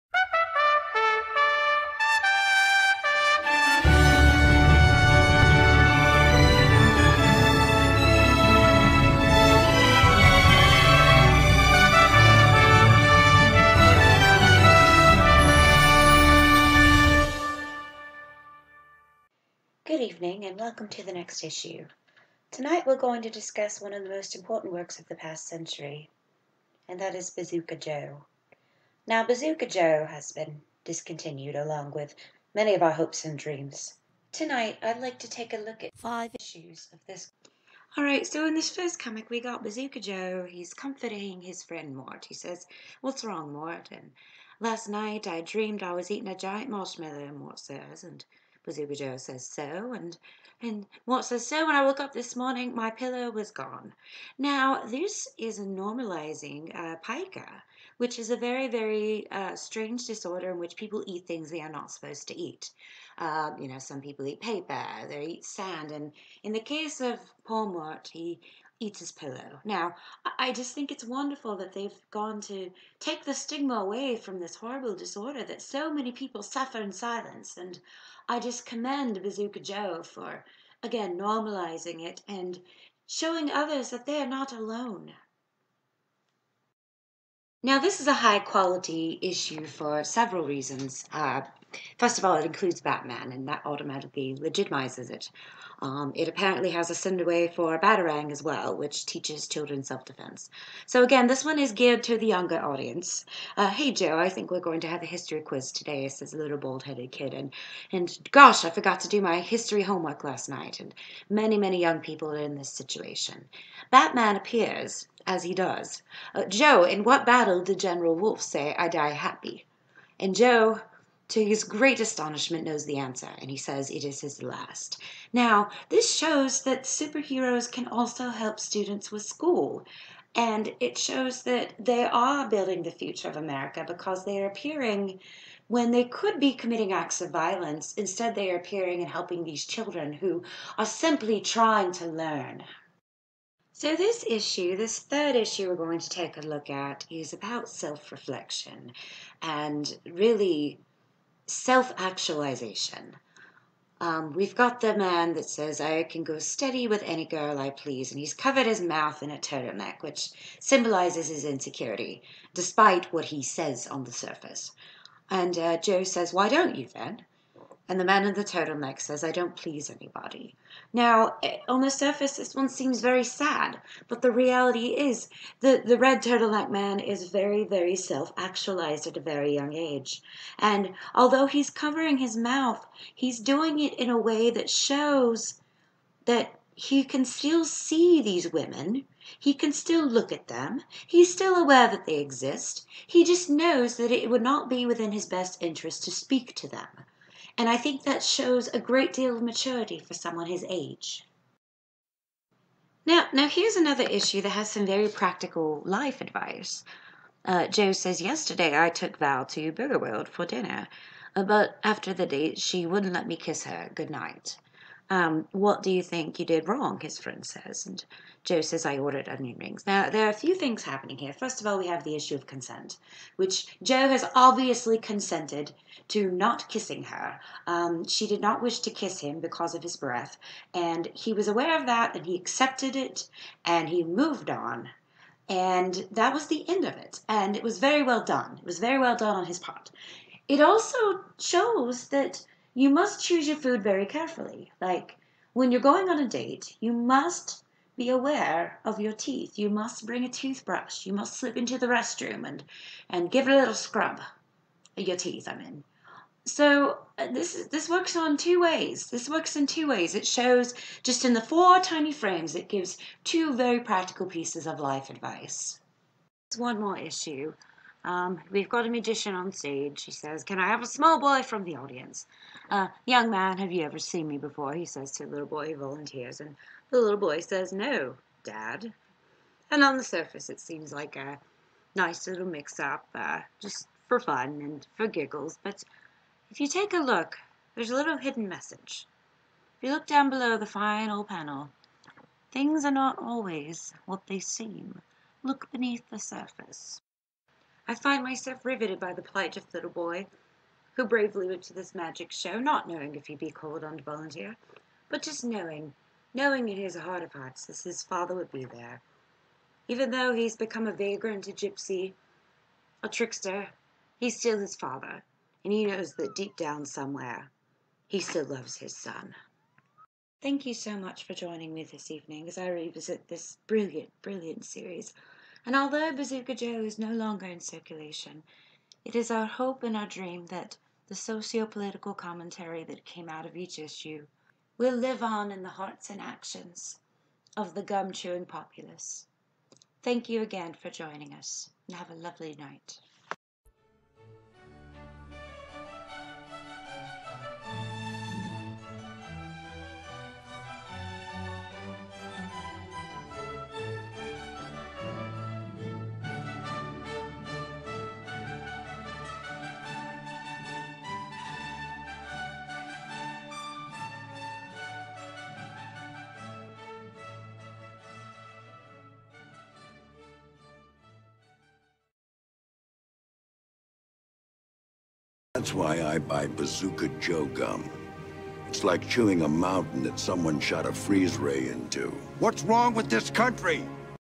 The Next Issue - Bazooka Joe
Today, we examine one of the finest comics ever printed, Bazooka Joe.  Don't let the price deceive you!  Many valuable comics have surprisingly low cover prices!

If you enjoy this video, be sure to check out the companion article